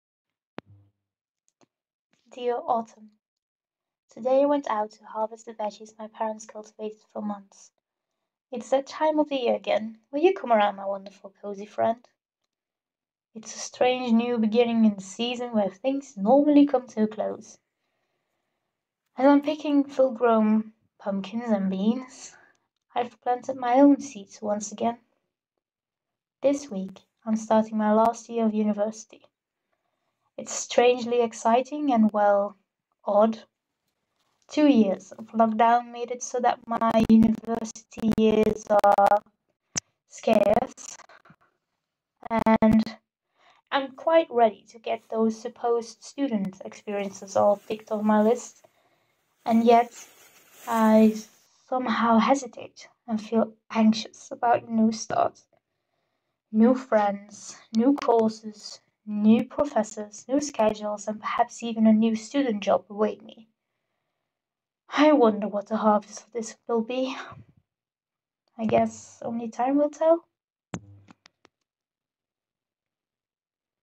Letters to Autumn 🍁 Reap what you sow
I'm trying something new in this season of new beginnings. I'm letting go of my fear of uploading something here.

This video (which will hopefully become a series of sorts) was inspired by a series of Carrie Hope Fletcher She is a wonderful creator, who I which I had as a friend.

I hope you enjoy this little letter and video. Have a nice autumn day. Until the next one?